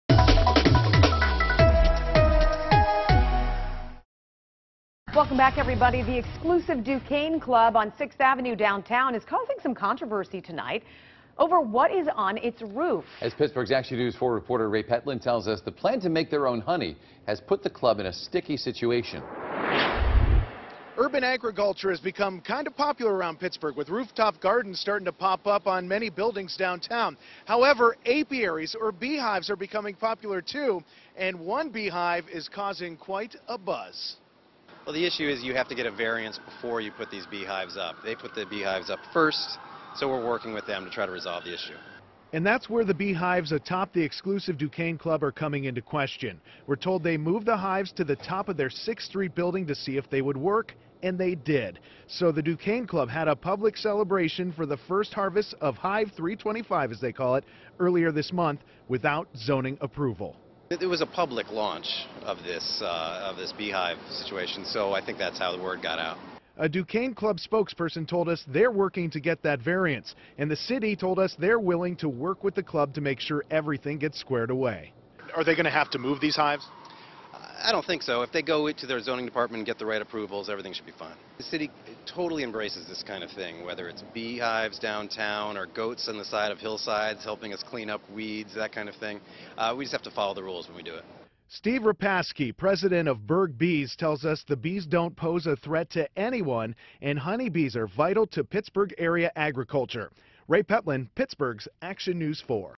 Duquesne Club hopes to keep bees buzzing at rooftop hives
Rooftop gardening has become popular at higher-end restaurants in Pittsburgh, as these dining establishments can tout restaurant-grown produce on the menu.